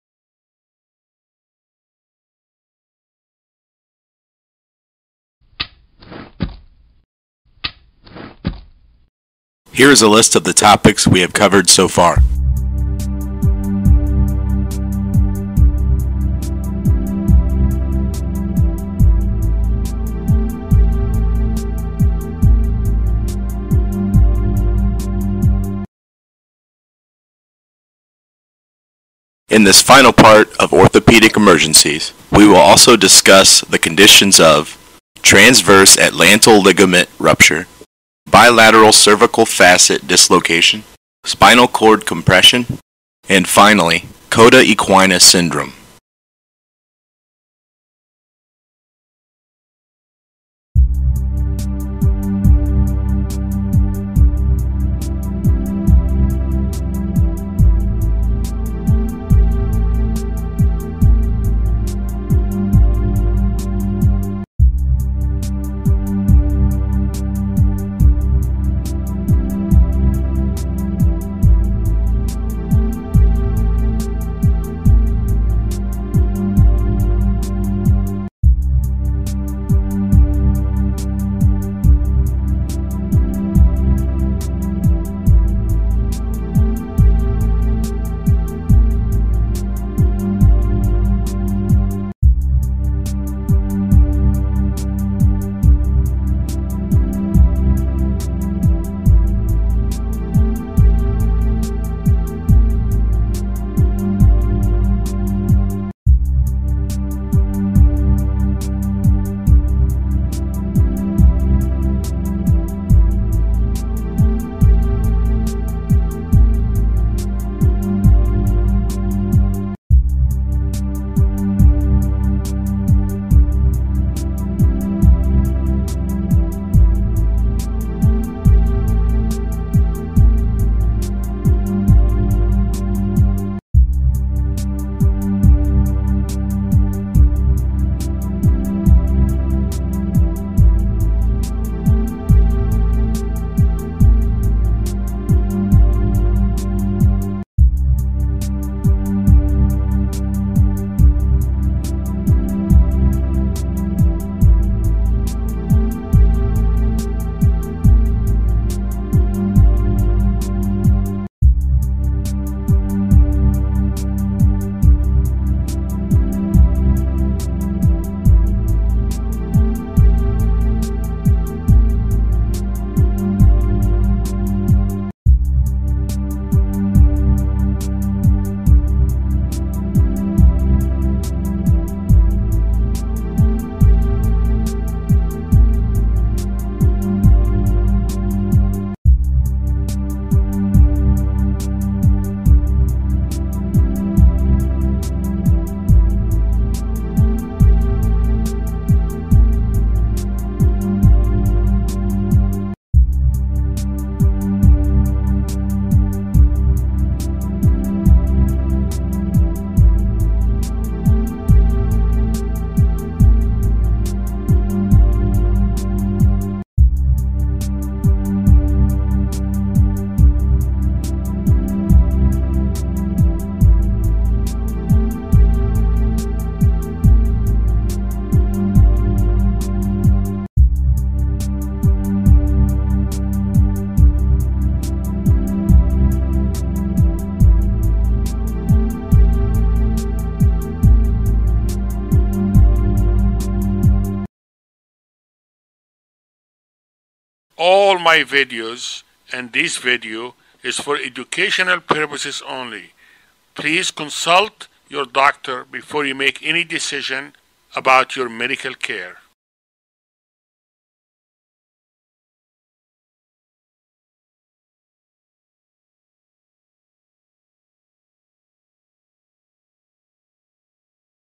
Orthopaedic Emergencies Part 4 - Everything You Need To Know - Dr. Nabil Ebraheim
Dr. Ebraheim’s educational animated video about Orthopaedic Emergencies.
Transverse Atlantal Ligament Rupture – translationally unstable in the sagittal plane due to transverse atlantal ligament rupture. It is usually apparent on x-rays or CT scan. If the condition is not diagnosed it can usually result in spinal cord compression, respiratory arrest and a catastrophic outcome. Usually it requires posterior atlanto-axial arthrodesis.
Facet dislocations of the cervical spine include unilateral facet dislocations and bilateral facet dislocation. For a unilateral facet dislocation the displacement is less than 50% of the vertebral body width and may need surgery. A bilateral facet dislocation displacement is greater than 50% of the vertebral body width. Usually needs surgery and make sure to exclude a disc herniation. Obtain a pre-operative MRI to rule out disc herniation associated with facet dislocations.
Spinal cord compression is more common with cervical spine injuries and thoracic spine injuries. Neurogenic shock resulting from spinal cord injury may complicate resuscitation of the patient and should be differentiated from hypovolemic shock. Look for hypotension and bradycardia. Look for thoracolumbar fractures, they may be missed. Emergency management involves resuscitation and hemodynamic stabilization with concurrent neurologic examination. Steroids are given early according to the protocol. The definitive treatment is stabilization of the unstable spinal injuries.
Cauda Equina Syndrome results from injury to the lumbosacral nerve roots within the spinal canal. It presents with involvement of the bladder, bowel, and lower limbs and usually results from central disc herniation or fractures. Central disc herniation or bony fragments result in compression of the nerve roots. Early diagnosis is imperative to find the cause of the compression on the nerve roots. Urgent decompression by the removal of the central disc herniation or stabilization of the fracture is necessary.